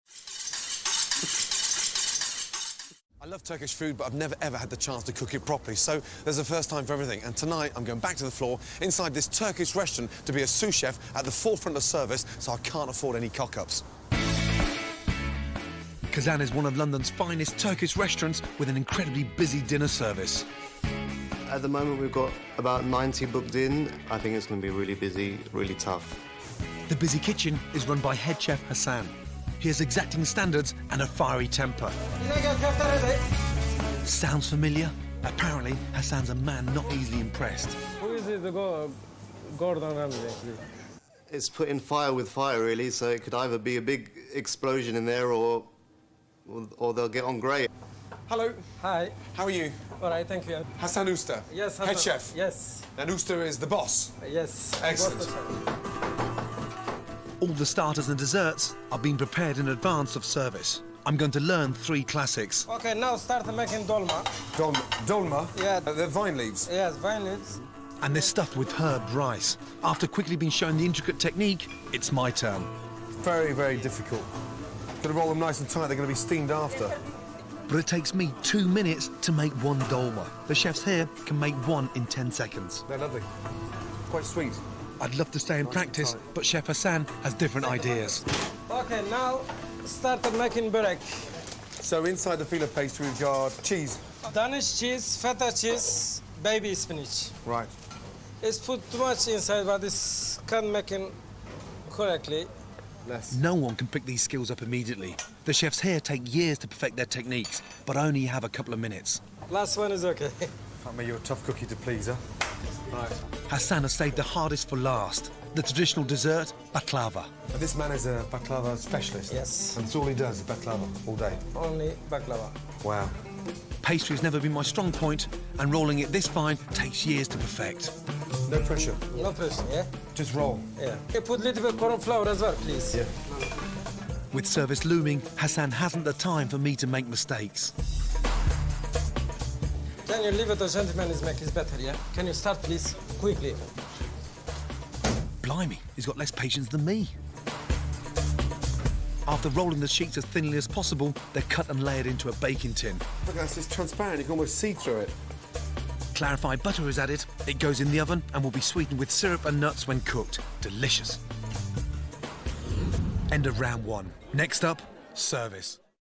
Türk Usta Gordon Ramsey'i Terletiyor (Türkçe Altyazılı)
Çeviri : Serra
En sevdiğim ve güldüğüm Gordon Ramsey videolarından biri.
Hasan Usta'nın klasik bir Türk şef olarak Gordon'u aşağılaması ve '' O kim ki ?'' demesi :D
Yorumlarınızı bekliyorum. Daha fazla Gordon Ramsay istiyorsanız lütfen yazın.
Bol yemekli karantinalar.